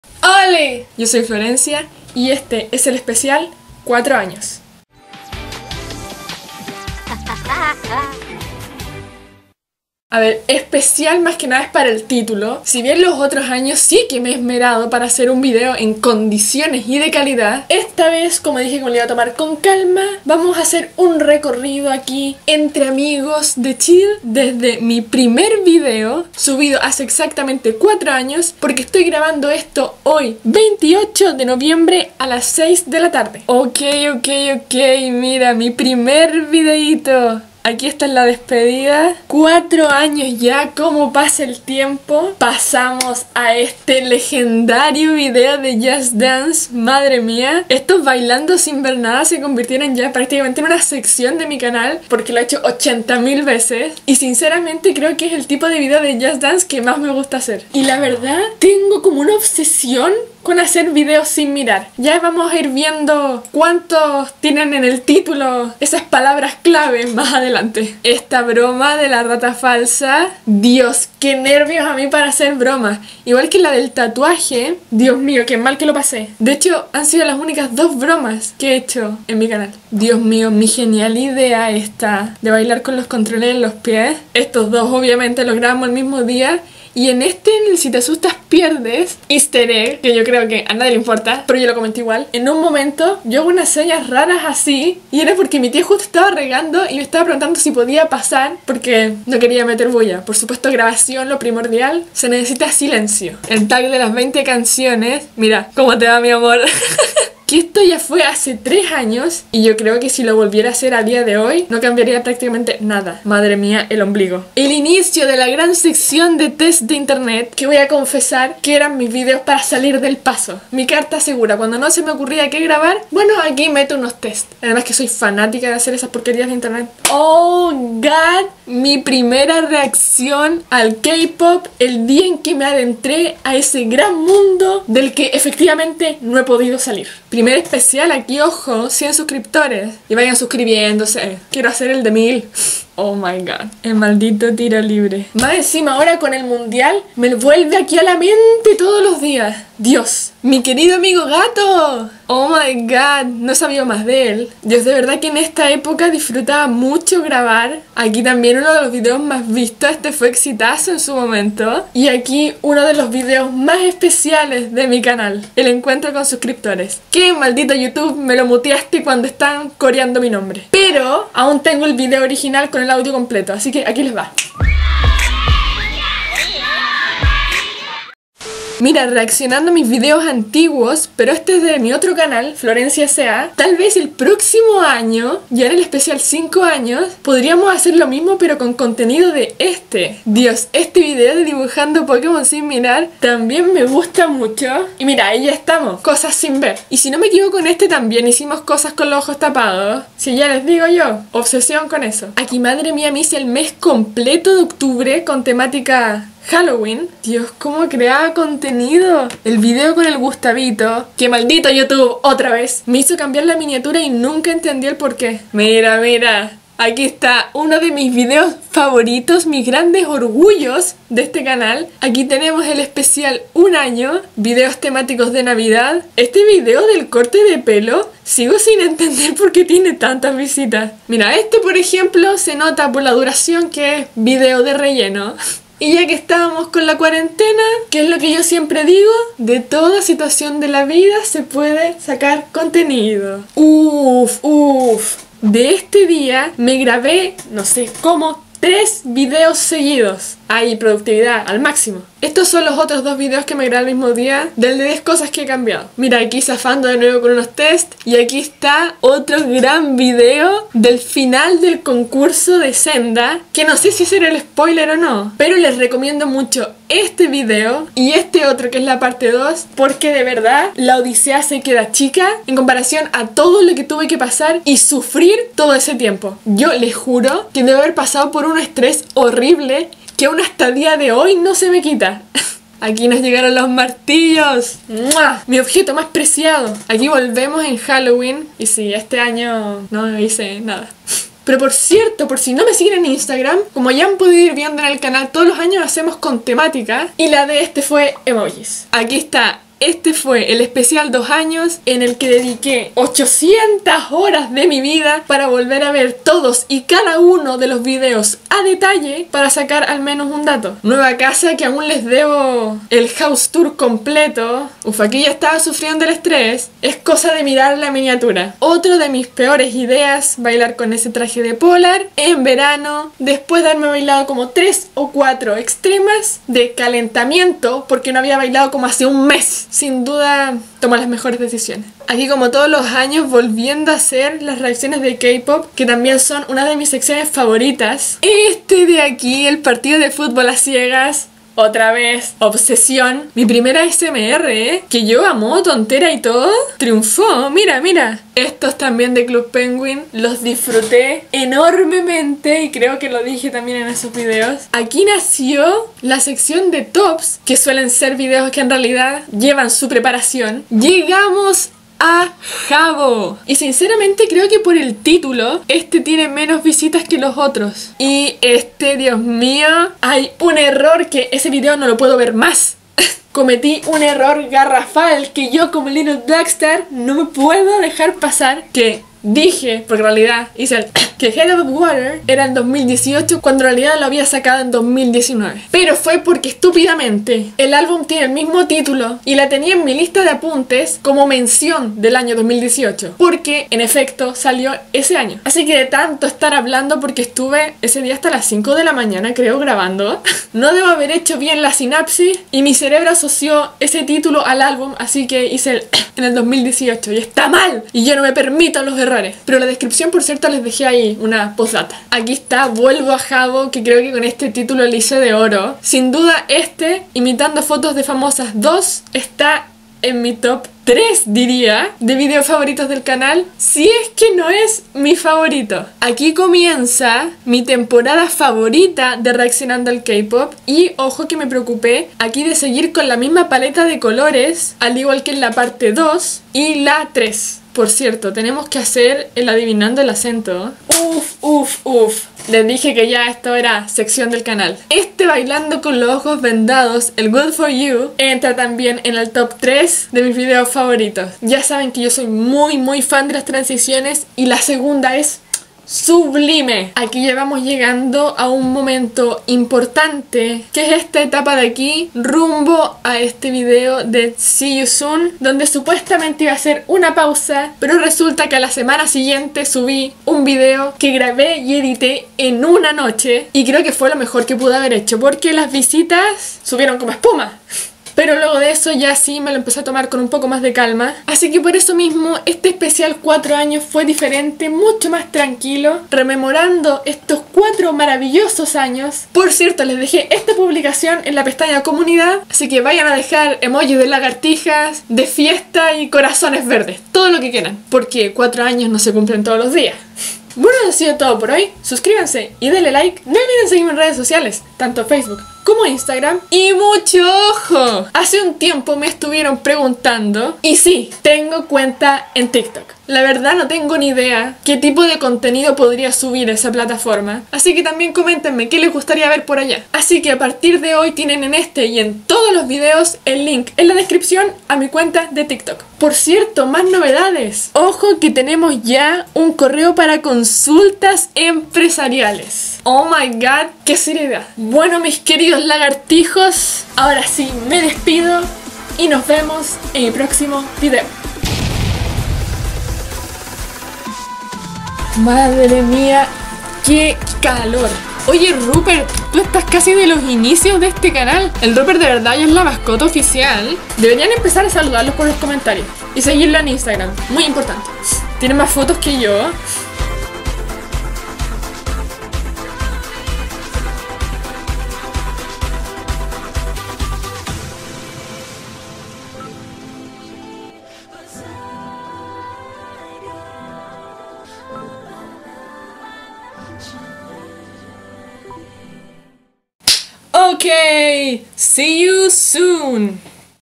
ESPECIAL 4 AÑOS EN YOUTUBE | I'm Florencia
*¡HOLI!* Hace 4 años subí mi primer video al canal, y para conmemorarlo, estaremos revisando y rememorando todo el contenido subido en este tiempo...